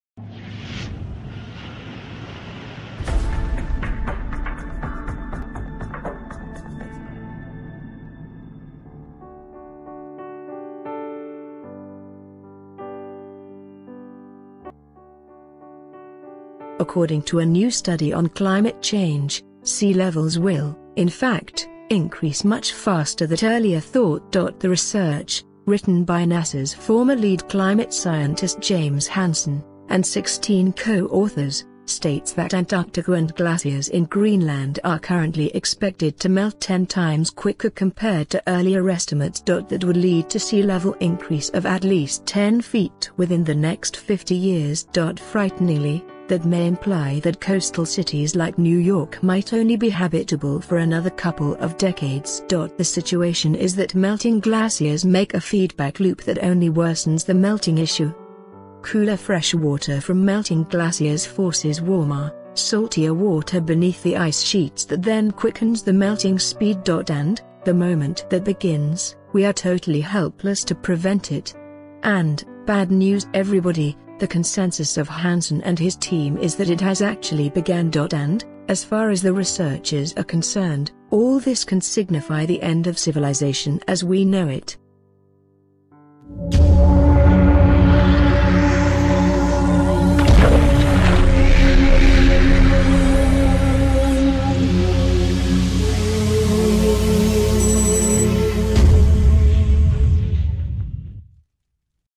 Scientists Say Global Warming Is Irreversible Now
According to a new study on climate change, sea levels will, in fact, increase much faster that earlier thought.

The research, written by NASA’s former lead climate scientist James Hansen, and sixteen co-authors; states that Antarctica and glaciers in Greenland are currently expected to melt ten times quicker compared to earlier estimates.
That would lead to sea level increase of at least ten feet within the next fifty years.

Frighteningly, that may imply that coastal cities like New York might only be habitable for another couple of decades.

The situation is that melting glaciers make a “feedback loop” that only worsens the melting issue. Cooler freshwater from melting glaciers forces warmer, saltier water beneath the ice sheets that then quickens the melting speed.

And, the moment that begins, we are totally helpless to prevent it. And, bad news everybody, the consensus of Hansen and his team is that it has actually began.

And, as far as the researchers are concerned, all this can signify the end of civilization as we know it.